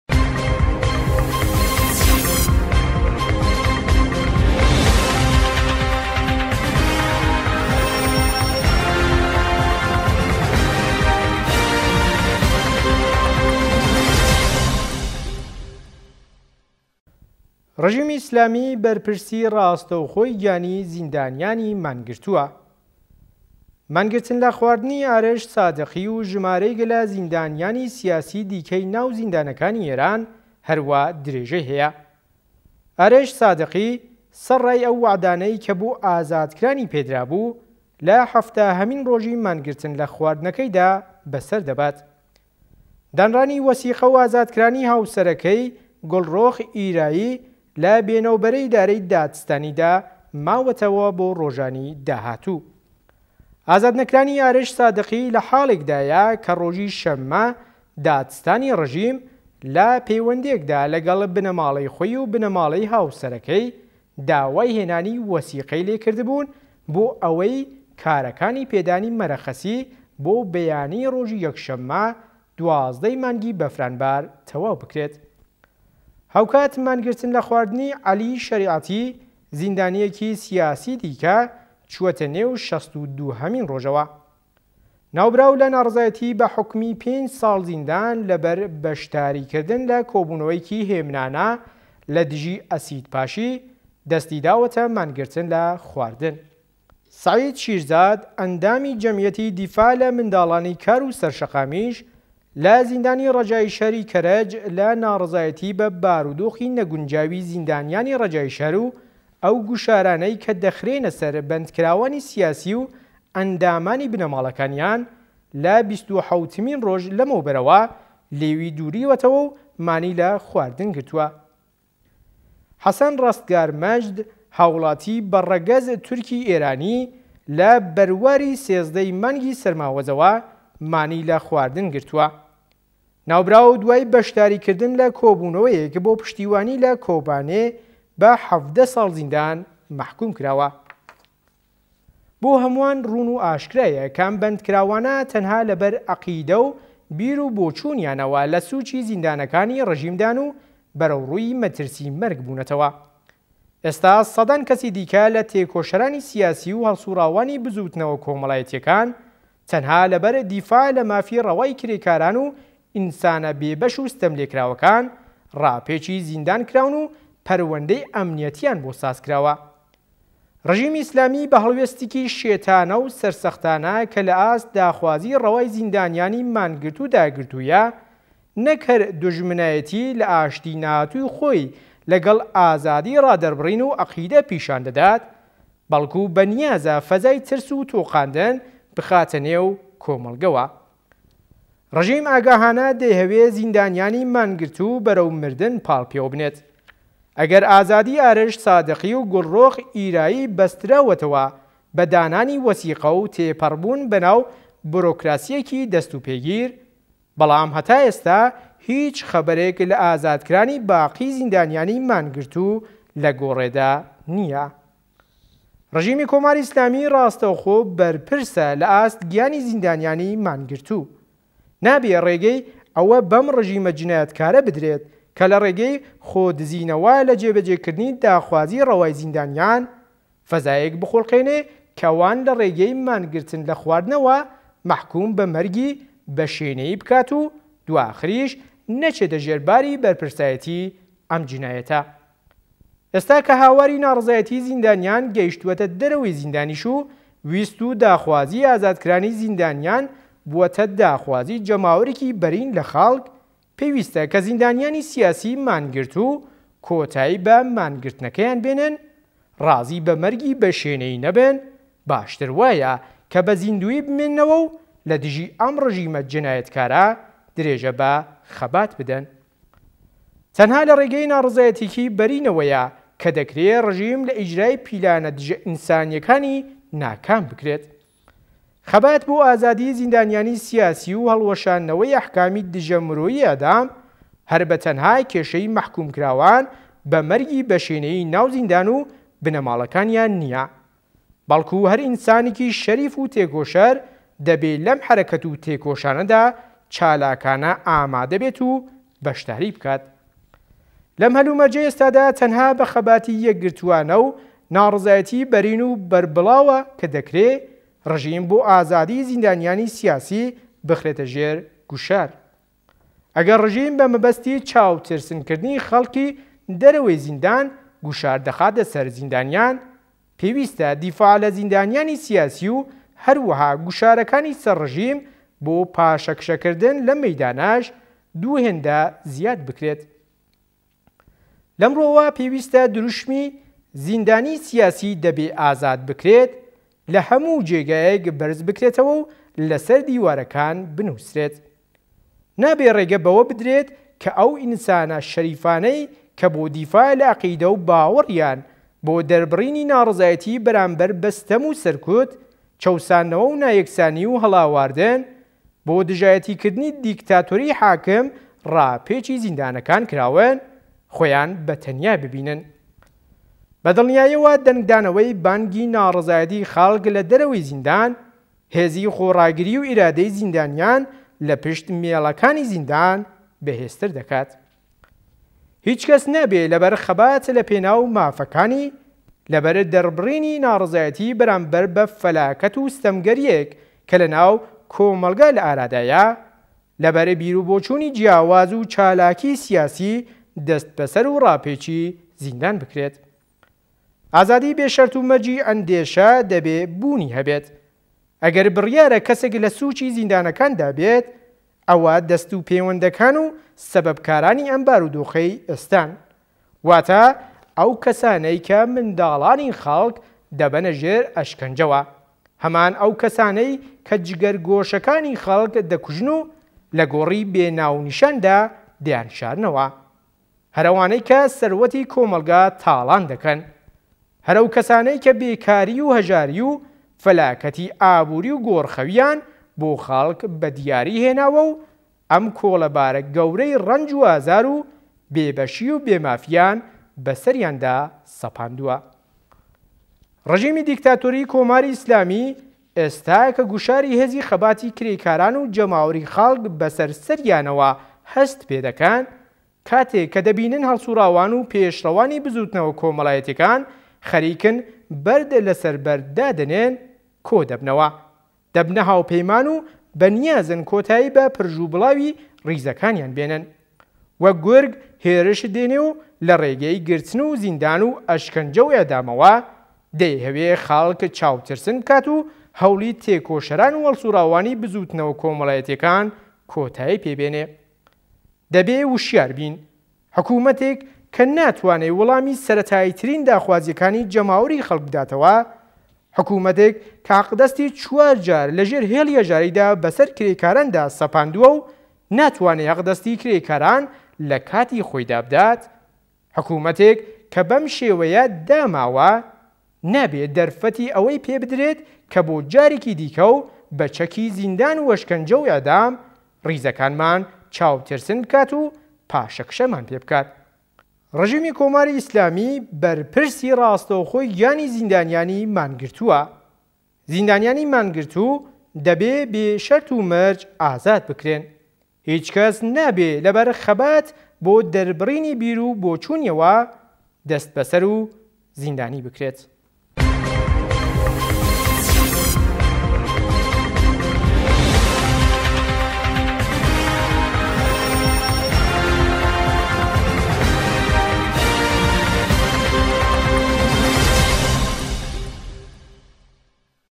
وته ی روژ 13.10.95
ڕژیمی ئیسلامیی بەرپرسی ڕاستەوخۆی گیانی زیندانییانی مانگرتووە